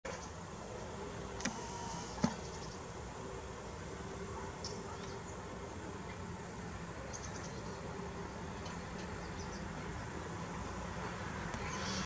12.08.2019. debesis virs Valmieras
Šodien plkst.17:11 mums gāja pāri šitāds !
Valmieras pagastā